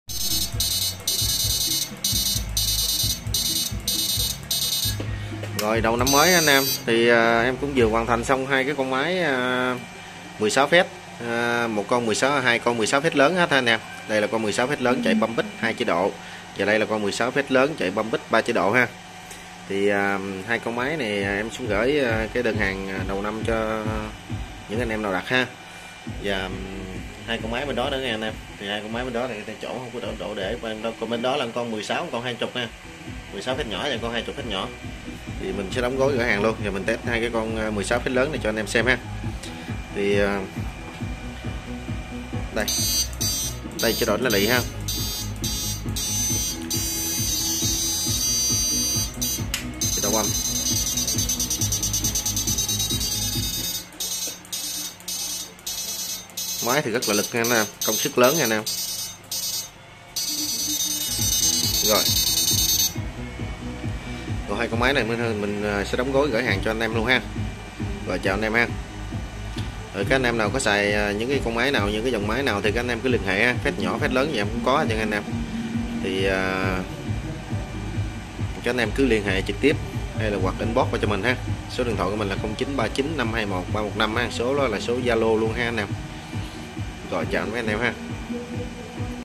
Gửi 2 máy 16 fet lớn đầu năm 2023 ae quan tâm LH Zalo 0939521315
Lộc đầu năm 2023 của em
Gửi  máy cho ae nhe đã đặt hàng trong tết
👉👉2 máy 16 fet lớn bâm pic chuyên thuyền
Ae quan tâm inbox LH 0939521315